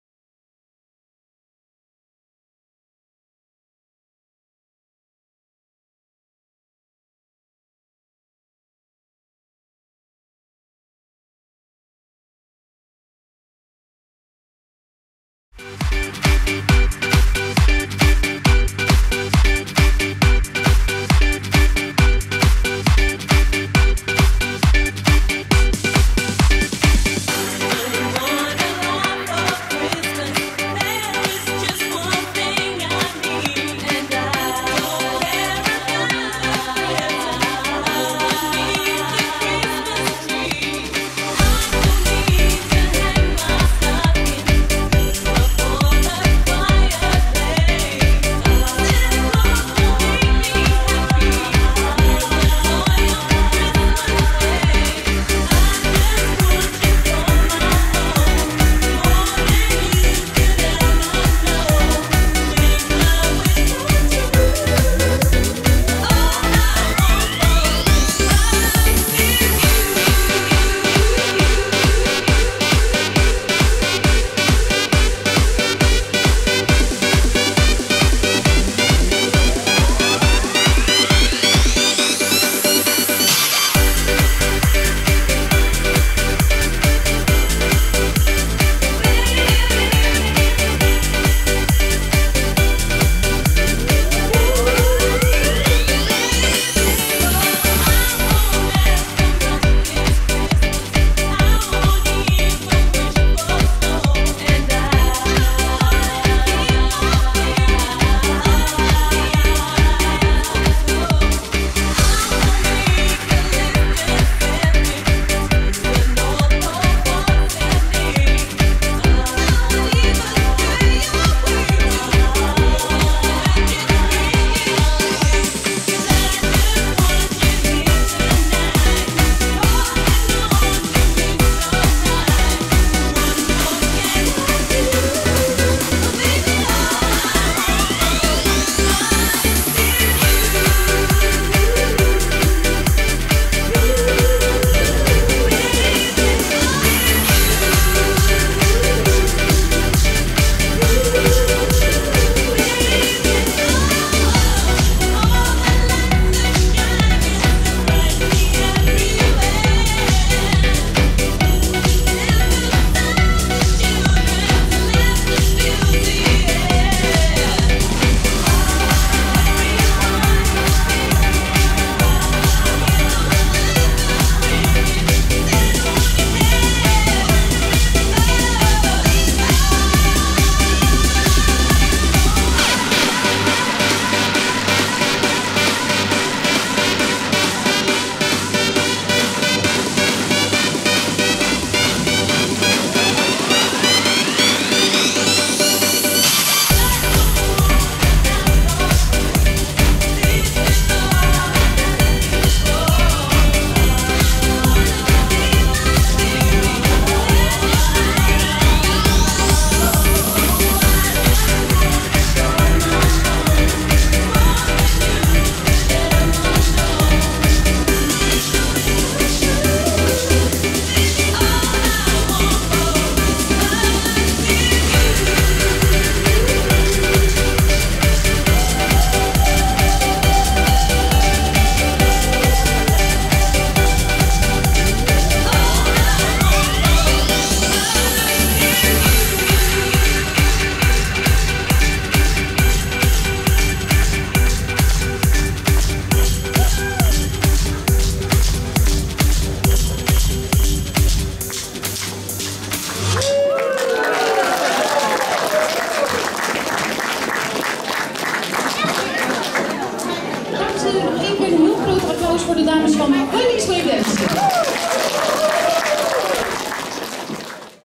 Optreden Wendy's Way Dancing tijdens Winter Christmas Vibes.
Tijdens de Winter Christmas Vibes in Zaal OEV in Millingen waren er optredens van Wendy's Way Dancing.